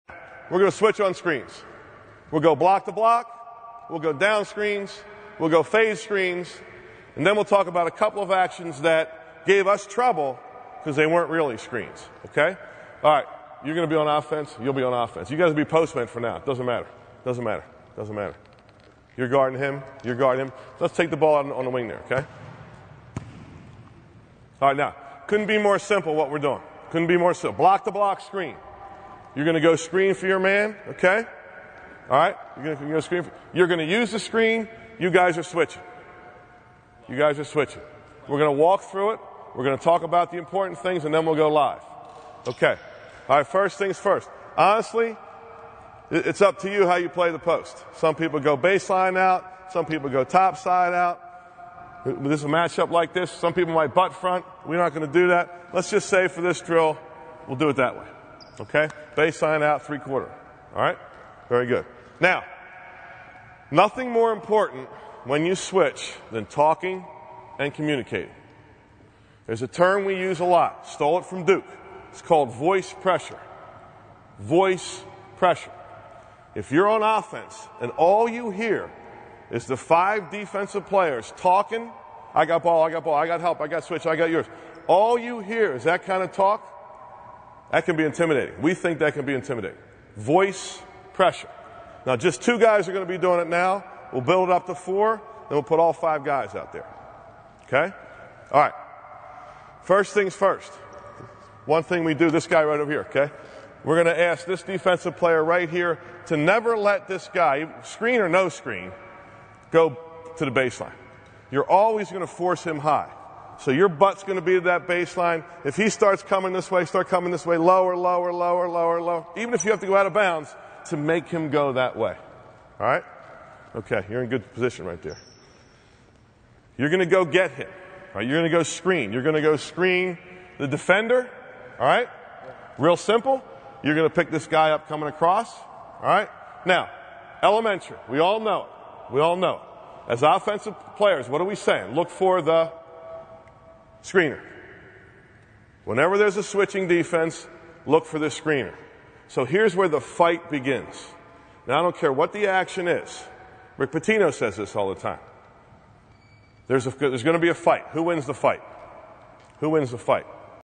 Block to Block Screens & Voice Pressure With Joe Mihalich
Joe Mihalich, the Niagara University basketball coach, demonstrates Block to Block Screens and Voice Pressure on the instructional basketball DVD Switching Man Defenses.

Since 1976, Championship Productions has been the world's #1 producer and distributor of instructional DVDs for coaches and athletes!